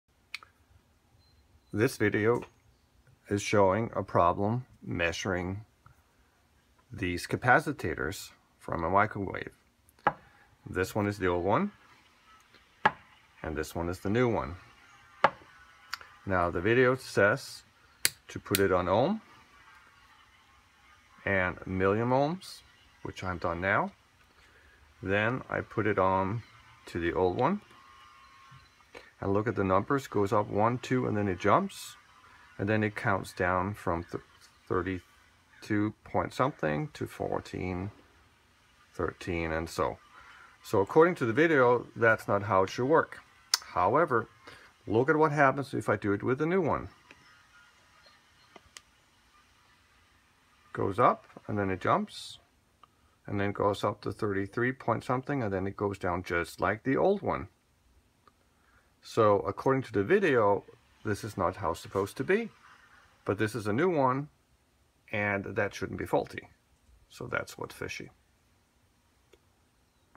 Testing problem for a capacitor
I am testing a capacitor like described by electronicsNmore in the video Microwave Oven Troubleshooting in MINUTES ~ STEP BY STEP but I do not see the Ohms go slowly up from 5M to 10M like expected, so I am wondering if your test method is consistent with all capacitors and Multimeters. I use a Triplett 9045 Multimeter to test the Capacitor (prod. no. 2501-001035). When measuring the Capacitor, the Multimeter jumps quickly within one or two second through the 2-5M range, then reads 0.L for a couple of seconds, and then comes on at 45 and then drops quickly to 15M, whereafter it goes down slowly towards 10M over a longer period. Based on what you say, this should be a sign that it is defective. However, I have bought a new Capacitor which when measured right out of the box does exactly the same even through it is brand new. I have also tested with another Digital Multimeter which gives the same results.  So the question is now how sure you are that your recommended test method is solid for all setups, or if the readings should be interpreted differently when measured on an Automatic Digital Multimeter. I think the likelihood that both my old and the new capacitator is defective is very slim.